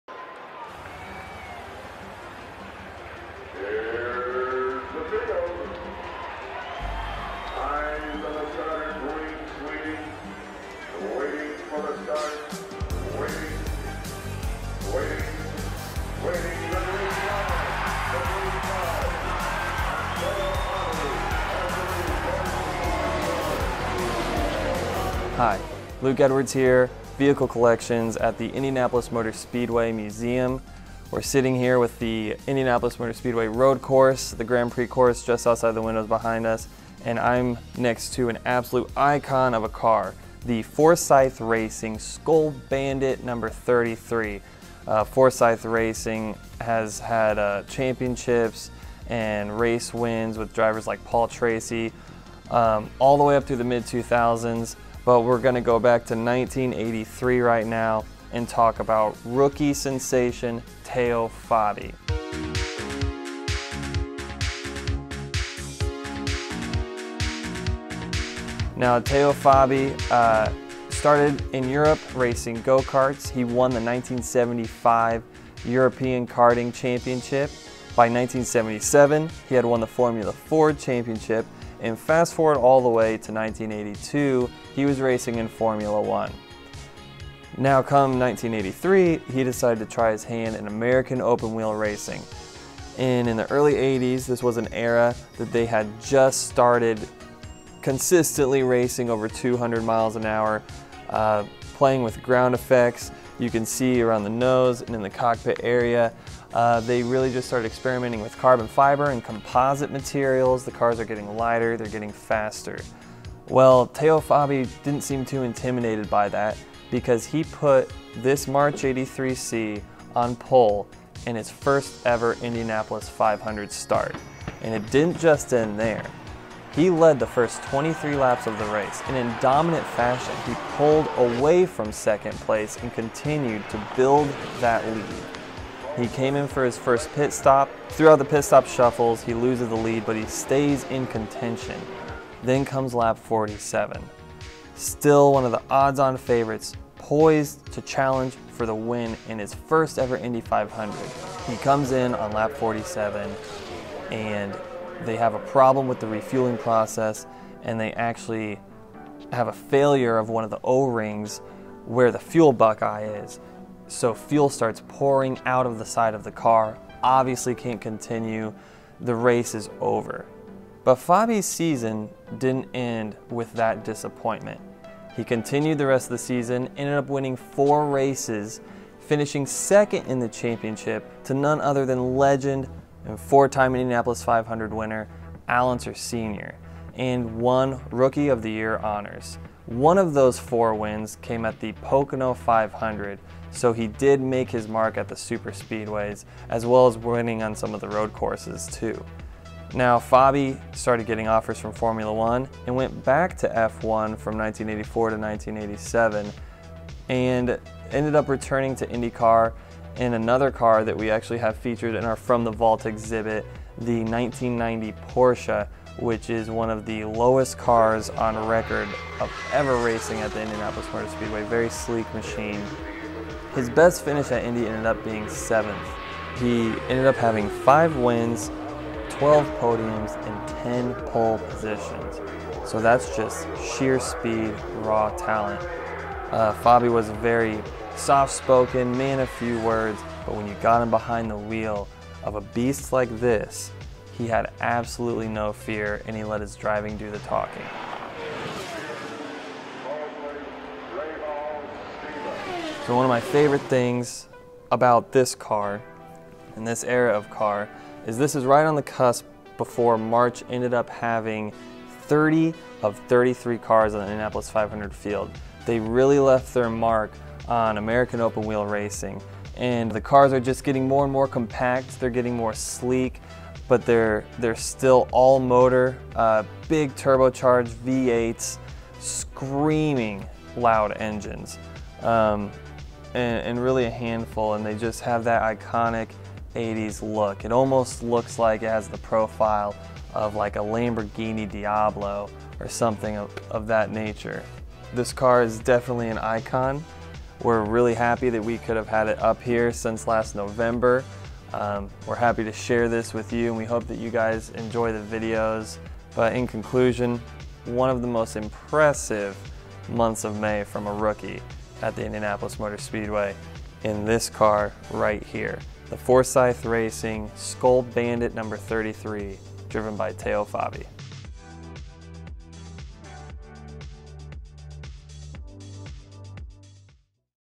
Featured Car Ep. 4 - Teo Fabi's 1983 March 83C
Vehicle Collections Specialist Luke goes back to 1983 - fast rookie Teo Fabi and his turbocharged Cosworth V8 March 83C, the duo that stole the show.

This car is part of the From the Vault exhibit presented by Bank of America